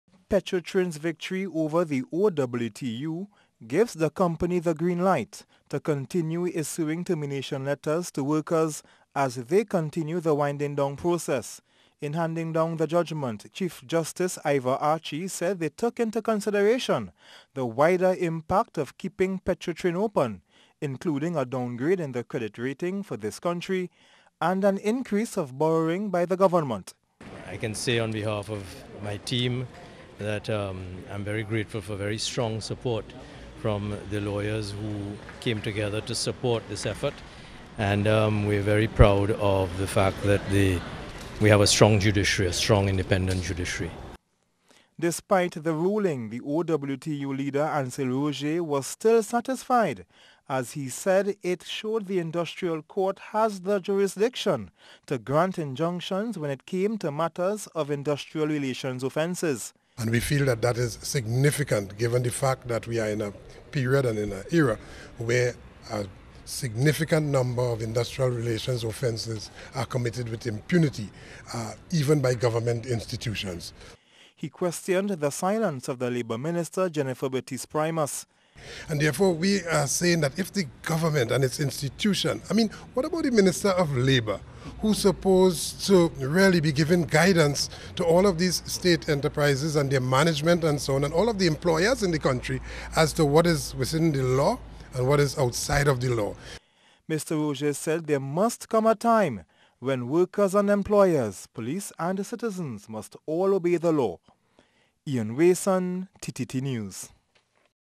Lifting Of Petrotrin Injunction Shows Strong And Independent Judiciary
Petrotrin's Attorney, Senior Counsel Reginald Amour, says Thursday's ruling by the Court of Appeal shows that the Judiciary is strong and independent.

On Thursday, the Court of Appeal, comprising Chief Justice Ivor Archie, and Justices Mendonca and Des Vignes, ruled in favour of Petrotrin and dismissed the injunction granted by the Industrial Court to the Oilfields Workers’ Trade Union that stopped Petrotrin from terminating workers.

Ian Wason has more.